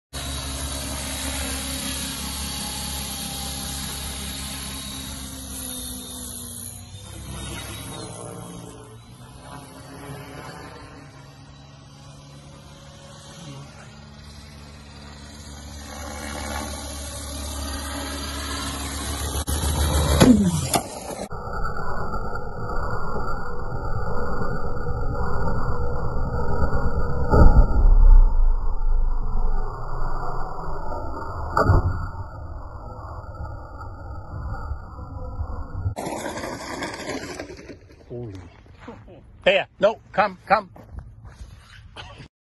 RC helicopter crash
first flight on my copter x 600 ends up exploding mid flight, most likely due to a linkage rod falling off.. enjoy!